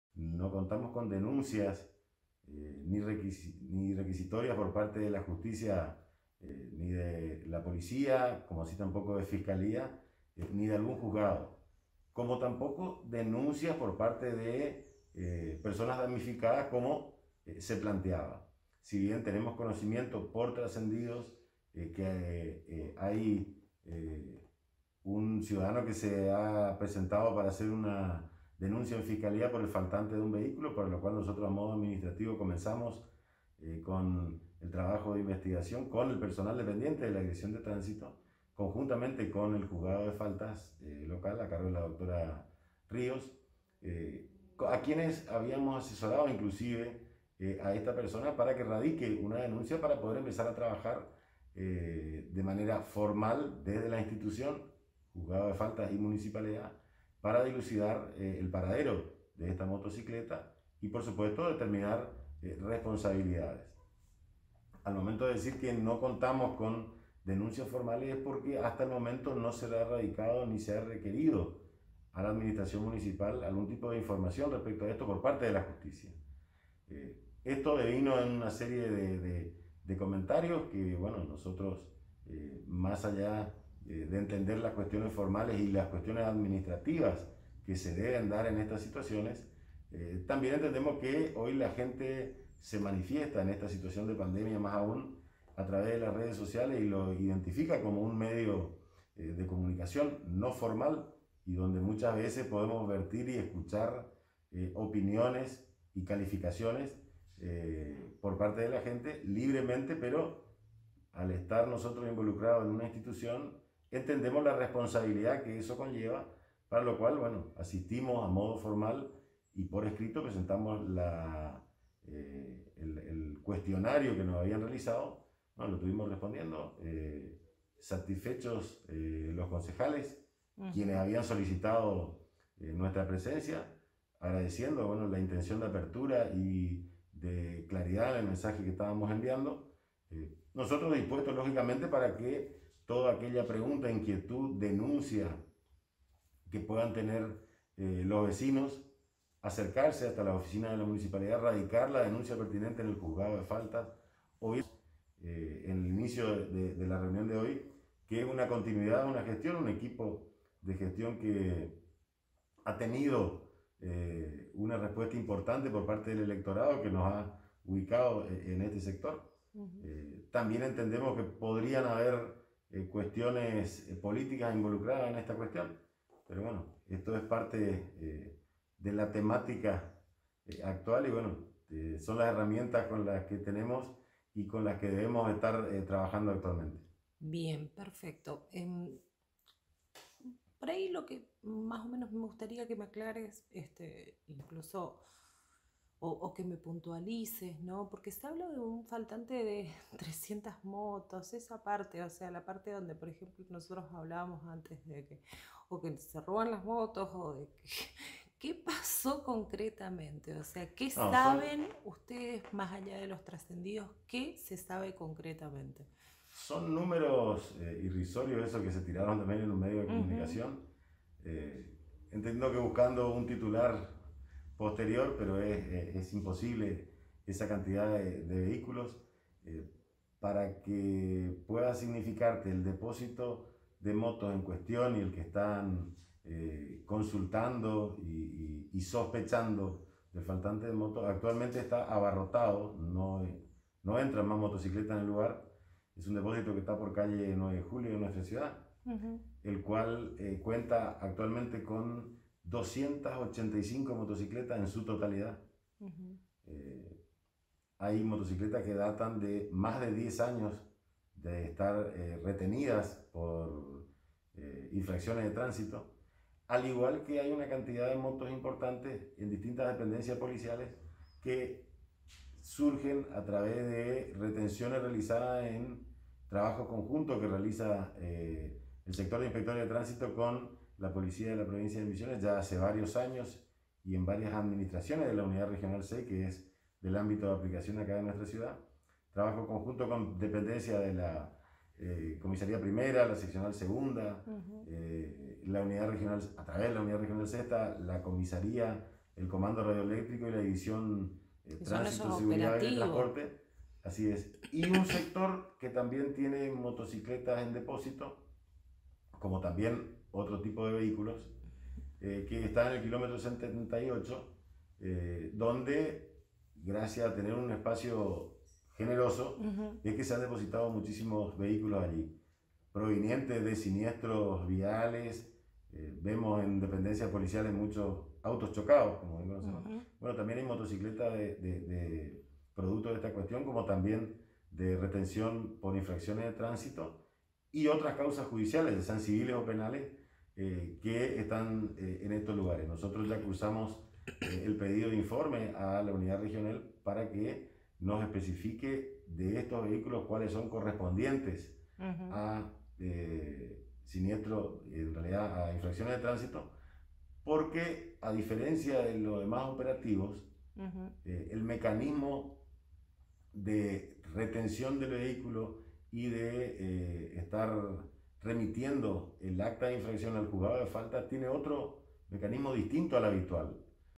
"No recibimos ninguna denuncia formal", aseguró el secretario de Gobierno de Alem
El secretario de Gobierno de la Municipalidad, Pablo Comby, indicó que asistieron a la sesión extraordinaria junto a la Directora de Tránsito, María Laura Wenk, para responder al pedido de informe, que fuera solicitado por los concejales del Bloque Cambiemos UCR.
El requerimiento de los ediles busca esclarecer un posible delito que involucraría a funcionarios y agentes municipales, vinculado al resguardo de vehículos en los depósitos municipales y que son retenidos por infracciones de tránsito. La presencia de los funcionarios en el recinto del HCD, fue para que los mismos suministren información que está en proceso investigativo.
“La interpelación fue para que informemos el mecanismo de trabajo que tenemos desde la Dirección de Tránsito Municipal” dijo Comby.
Aclaro que a “modo formal en la administración no cuentan con denuncias por parte de supuestos damnificados, ni requisitorias por parte de la justicia, ni de la policía, ni de fiscalía como así también de ningún juzgado.”